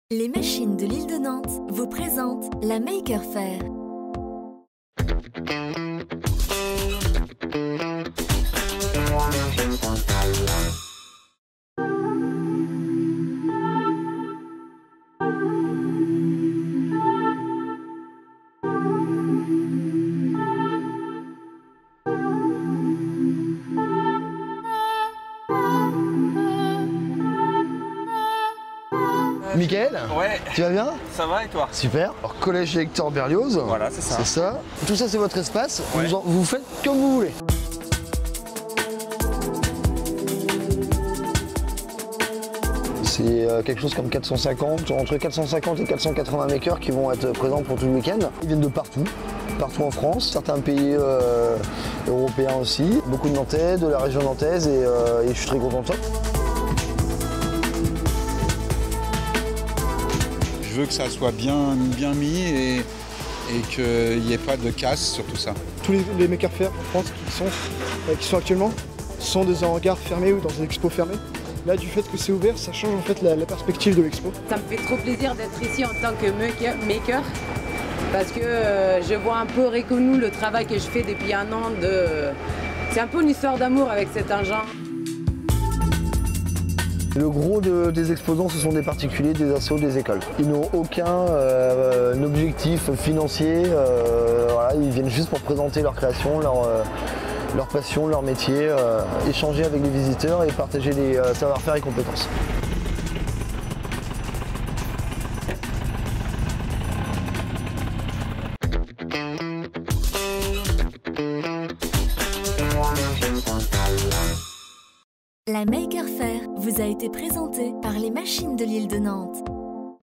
Les Makers : Jour 2
J-1 avant le début de la deuxième édition de la Maker Faire à Nantes ! Les premiers créateurs s'installent déjà sous les Nefs et sur le parc des Chantiers. Ils seront près de 500 ce week-end à présenter leur invention au public nantais.